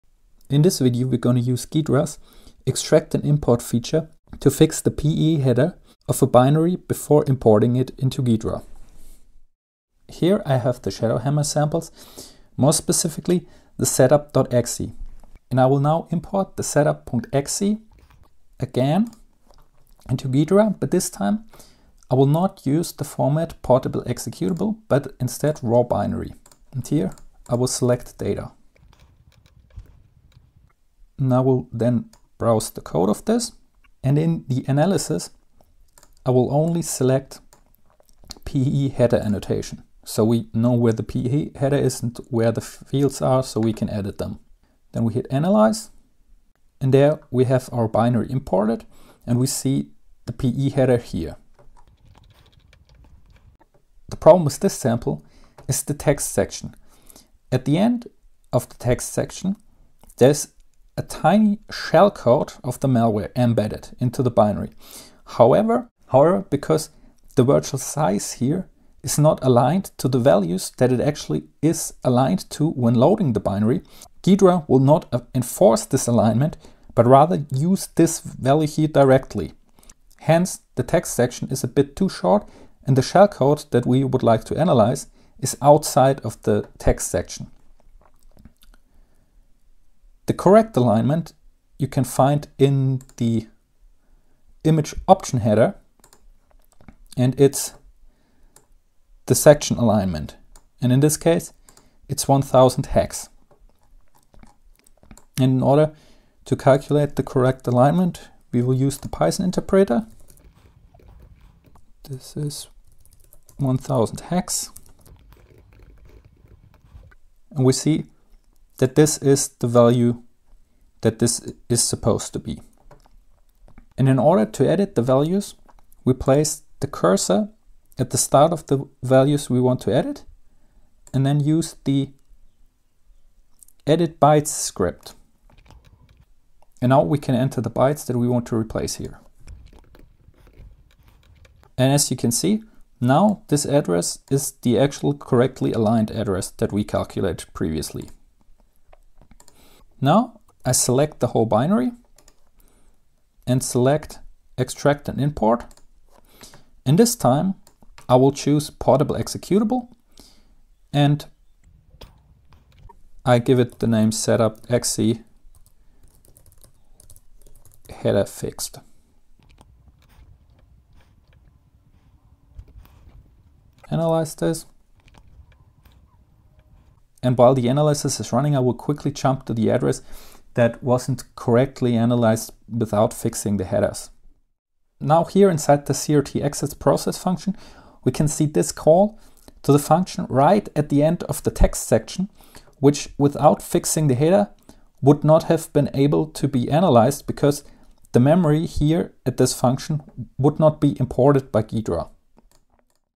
Ghidra: EditBytesScript (to fix/manipulate PE header to load ShadowHammer setup.exe sample)
Due to a bug in Ghidra some people (me included) were having trouble analyzing the ShadowHammer setup.exe sample with Ghidra.
With the EditBytesScript.java you can fix/manipulate the PE header, then re-import the fixed sample into Ghidra via "Extract and Import ...".

NOTE: I hope this PE parsing bug gets fixed in Ghidra. However, you can still use the EditBytesScript.java to fix/manipulate corrupted PE headers, or also other fields of binaries.